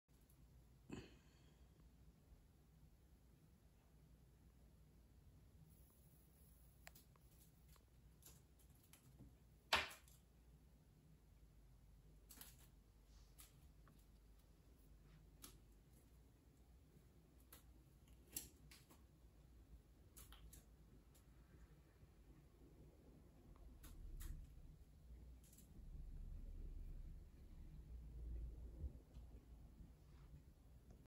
Fay speelt verder
Sherlock wilt daarna met haar spelen